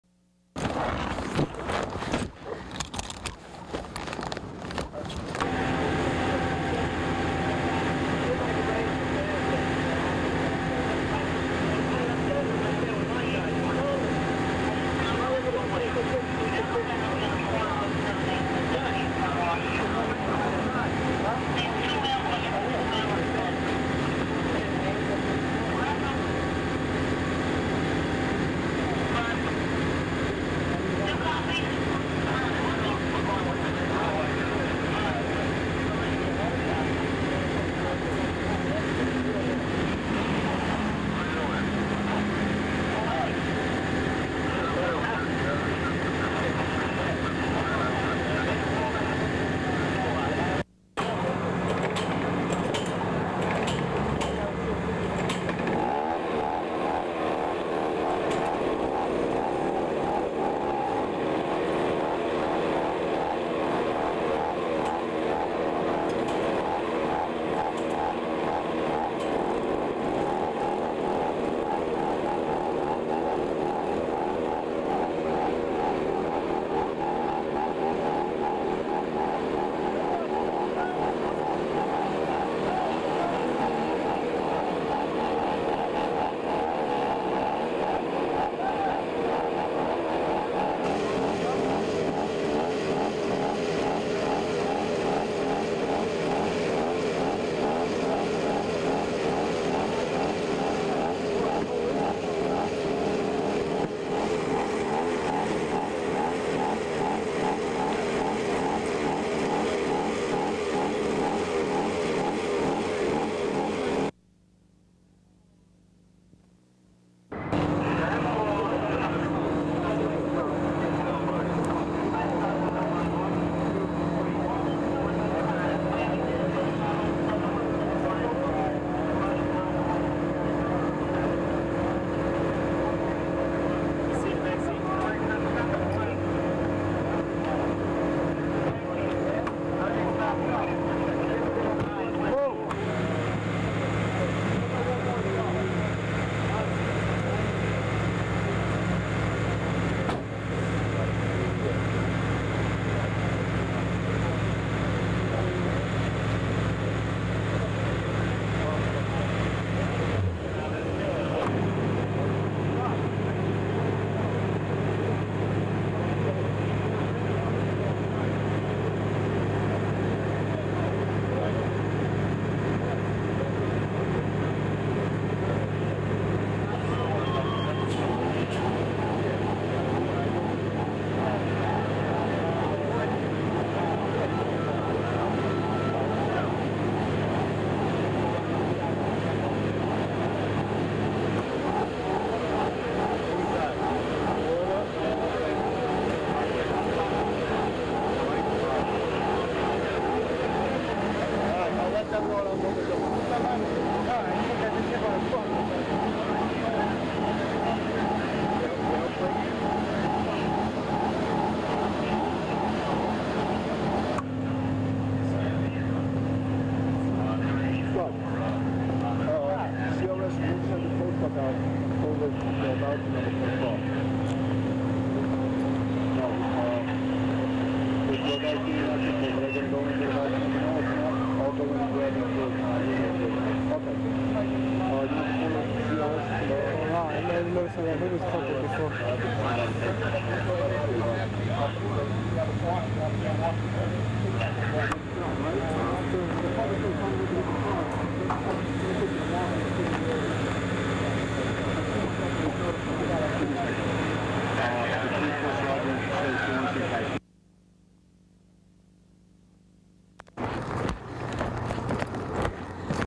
DOA ACCIDENT HENRY HUDSON PARKWAY, MANHATTAN - 1989
-CAR MISSING FRONT END WRAPPED AROUND TREE,NYPD COPS USE JAWS OF LIFE, HYDRAULIC SAW TO CUT CAR APART, HIGHWAY IN WASHINGTON HEIGHTS
-BODY OF VICTIM IS LOADED INTO AMBULANCE BY NYPD POLICE OFFICERS, BAD SINGLE CAR ACCIDENT, ACCIDENT INVESTIGATION AND CLEAN-UP, EXPOSED ENGINE IN PIECES